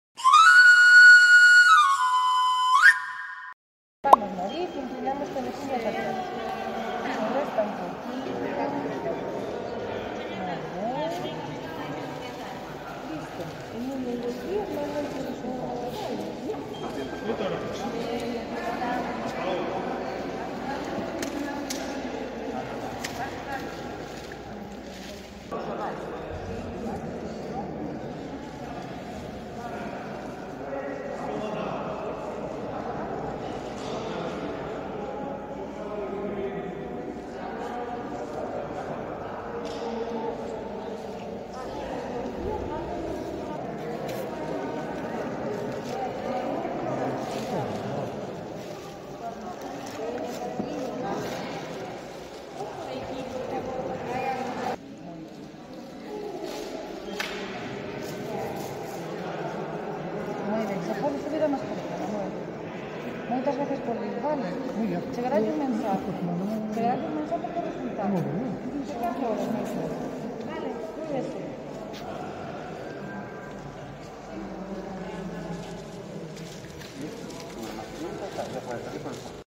Más de 380 personas acudieron al cribado de O Irixo
Al menos 389 personas de las 500 citas acudieron al cribado que Sanidade llevó a cabo este miércoles en O Irixo